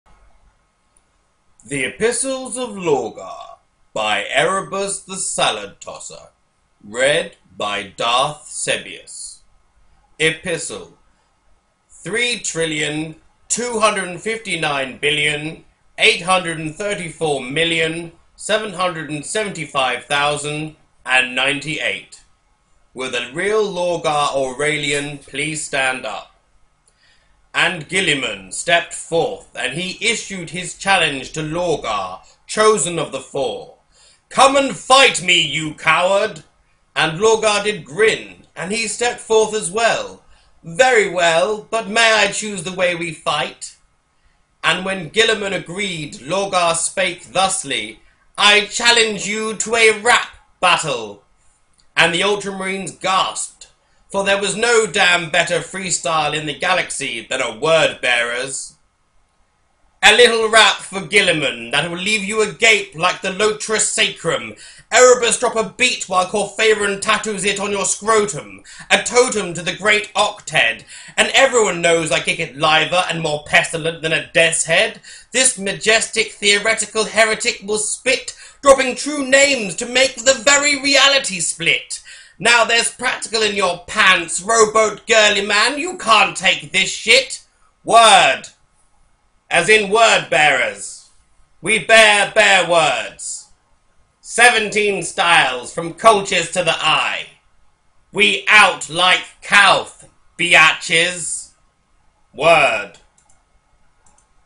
The Epistles of Lorgar - 3,259,834,775,98 (Will The Real Lorgar Aurelian Please Stand Up)
Epistle 3,259,834,775,98 of Lorgar (Will The Real Lorgar Aurelian Please Stand Up)

"And Guilliman stepped forth, and he issued his challenge to Lorgar, Chosen of the Four. "Come and fight me, you coward!"
And Lorgar did grin, and he stepped forth as well. "Very well. But may I choose the way we fight?"
And when Guilliman agreed, Lorgar spake thusly. "I challenge you to a rap battle."
And the Ultramarines gasped, for there was no damn better freestyle in the galaxy than a Word Bearer's."
A little rap for Guilliman that will leave you agape like the Latros Sacrum,
Erebus drop a beat while Kor Phaeron tattoos it on your scrotum,
 a totem to the great Octed,
and everyone knows I kick it liver and more pestilent than a Deaths Head,
This majestic theoritcal heretic will spit, dropping true names that make reality split,
now there's practical in your pants, Rowboat Girlyman can't take this shit
Word...as is Word Bearers
We bear bare words
17 styles from Colchis to the Eye
We out like Calth, biatches! WORD

The Epistles of Lorgar are the brainchild of ErebusTheSaladTosser and read by Darth Sebious